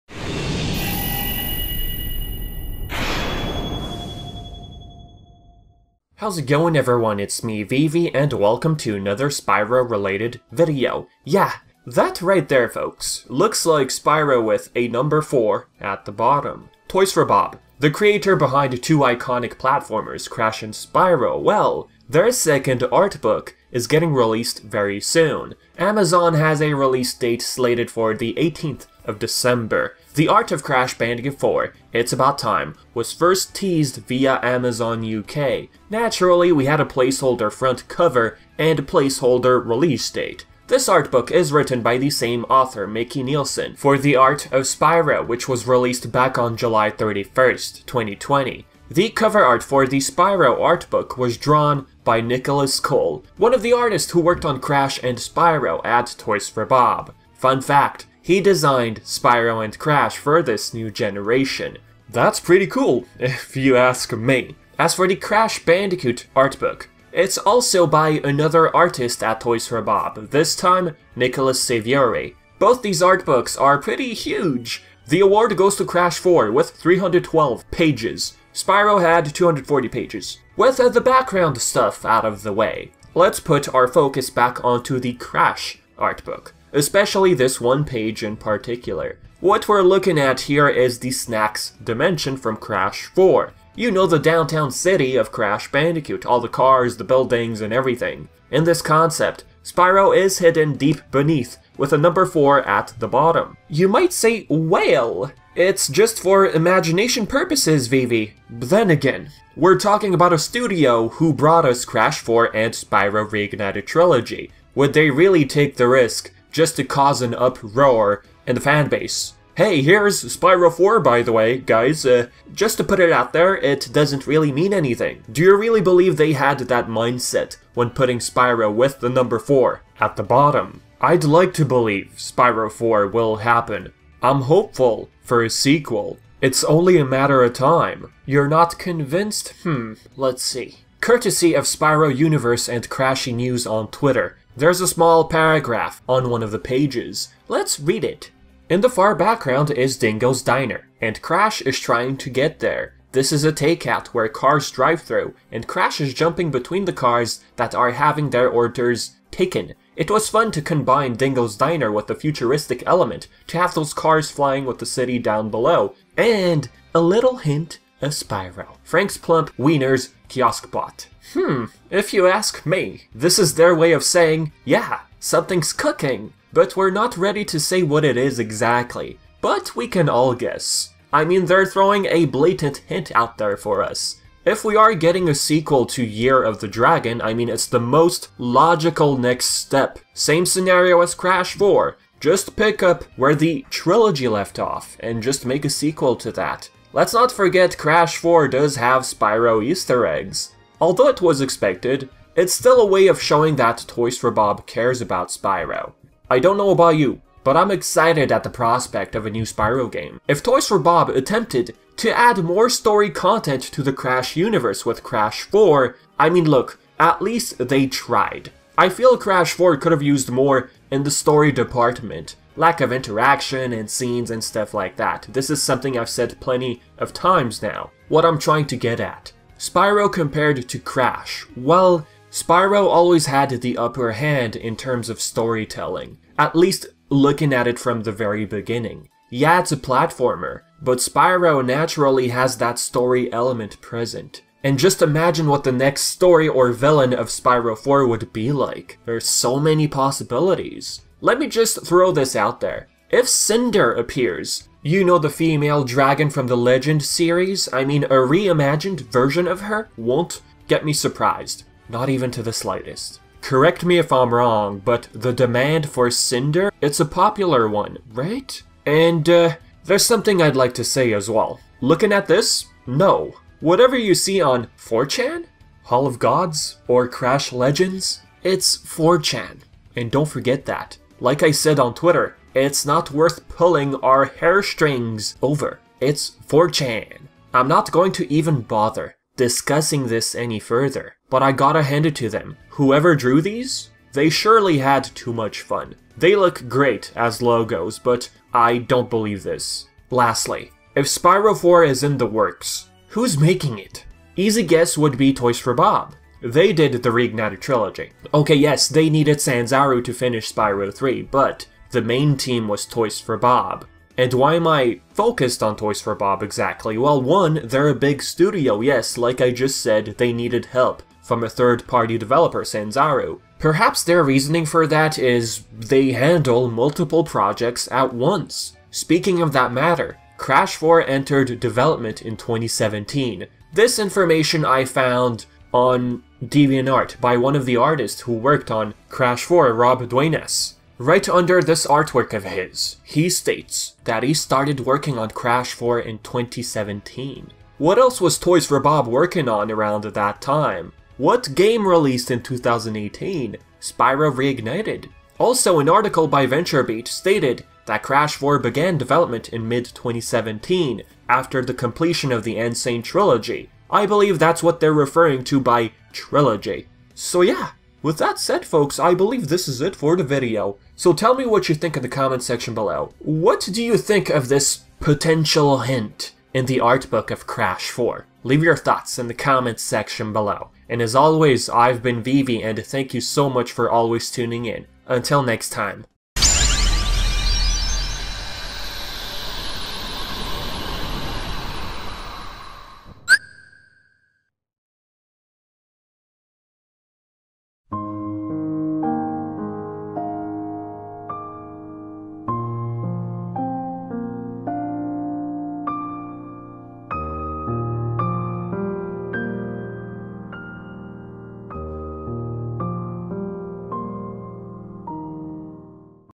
Is This A Possible Hint At Spyro 4?
The title ‘’Spyro 4’’ appears to be hidden on one of the pages of the upcoming art book for Crash Bandicoot 4: It’s About Time. Just like the Art of Spyro, Crash 4 is getting its own art book very soon! The soon-to-be released Crash Bandicoot art book seems to contain a ‘’little hint of Spyro’’.

Toys For Bob brought us the Spyro Reignited Trilogy in 2018. Their latest project was Crash Bandicoot 4: It’s About Time. Crash 4 apparently began production in 2017. One year later, Spyro the Dragon got its own remake.  What this tells us, Toys For Bob is capable of handling multiple projects at a time. Could the developer be working on a sequel to Year Of The Dragon ‘’Spyro 4’’?